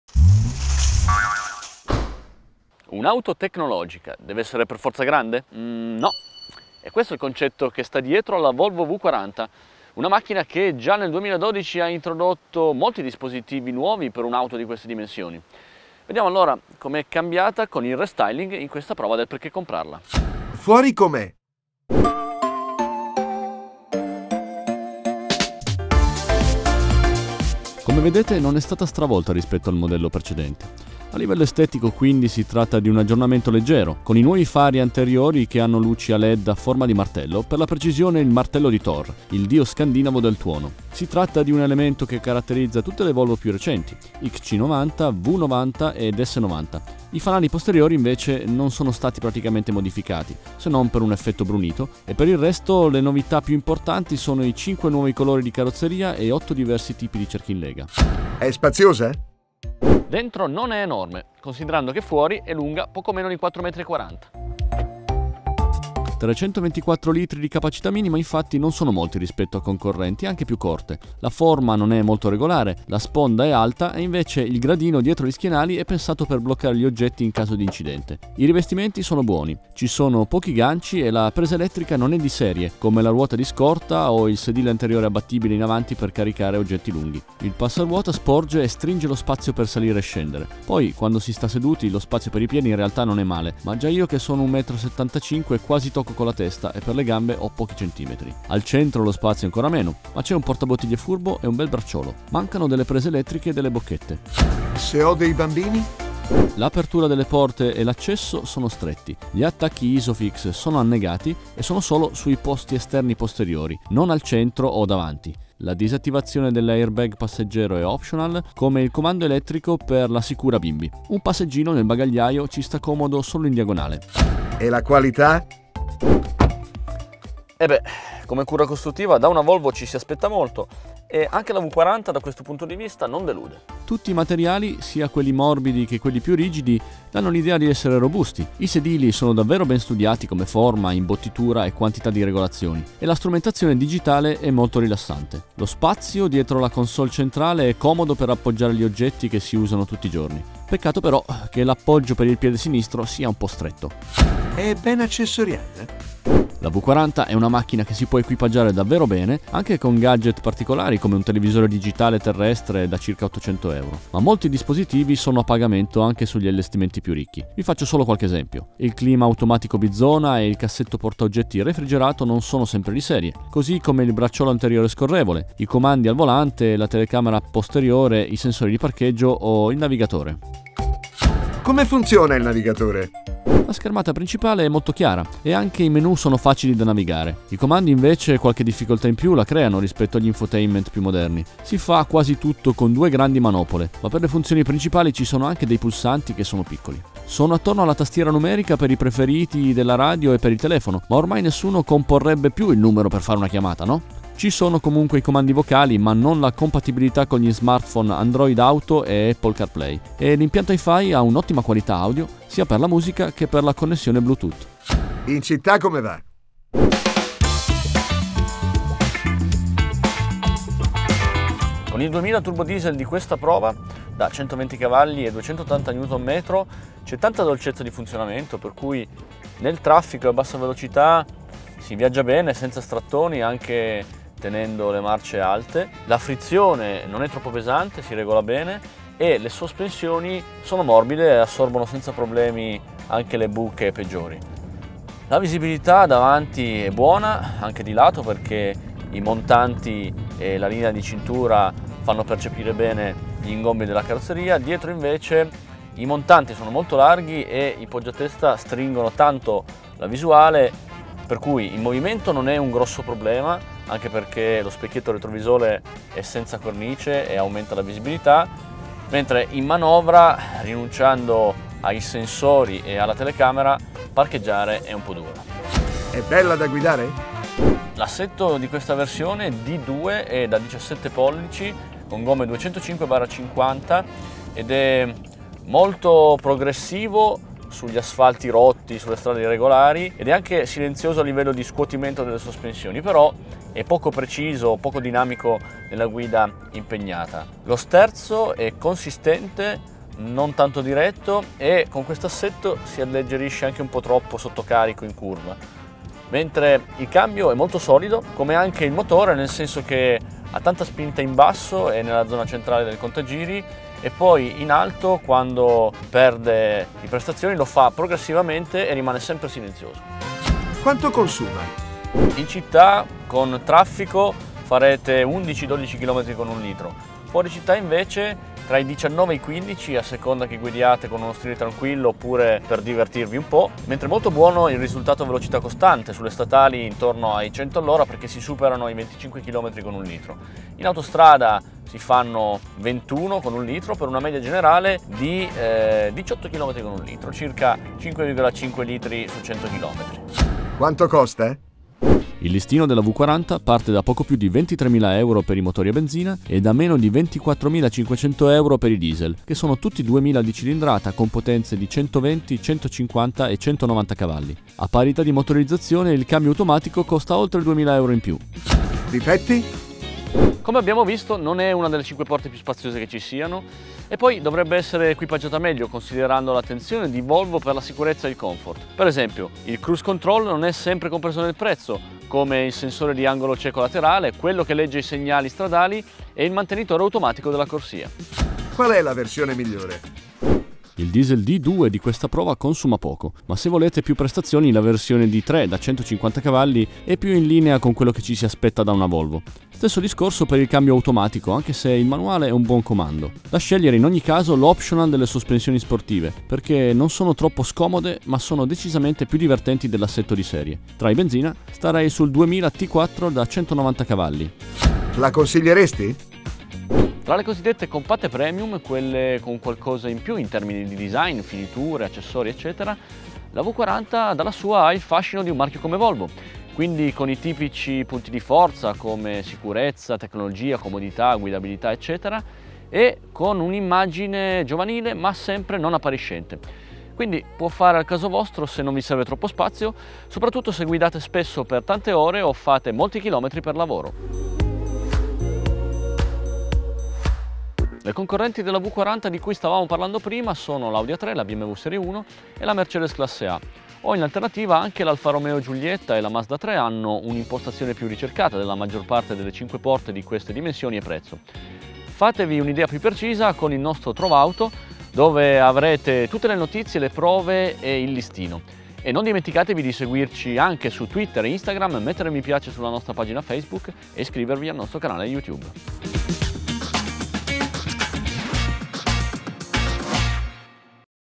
Volvo V40 restyling (2016) | Perché comprarla... e perché no
Non bisogna più comprarsi per forza un “macchinone” per avere un’auto di categoria superiore. Sono soprattutto le cosiddette compatte premium - e cioè macchine con marchi di prestigio, finiture ricercate e tecnologie all’avanguardia - a confermare questa tendenza e la Volvo V40 è un modello che appartiene proprio alla suddetta categoria: è lei la protagonista della nostra ultima prova del perchécomprarla, nella versione restyling che arriva sul mercato a quattro anni dal debutto. [Per saperne di più

CREDITS
Produzione: Edimotive